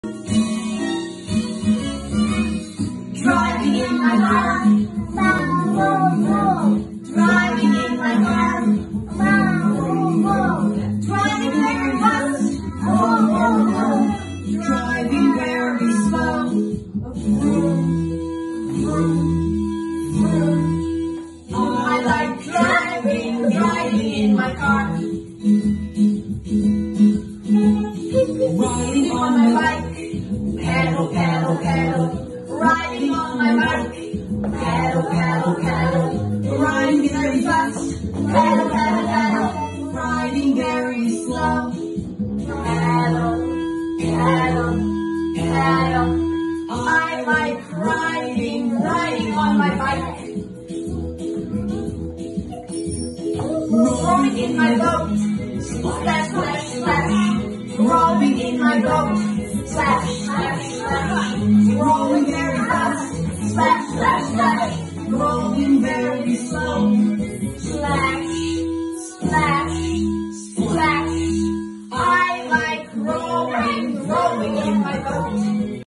Transport
Дітки молодшої групи на English lesson вивчали Transport 🚙🚆🚲⛵✈️.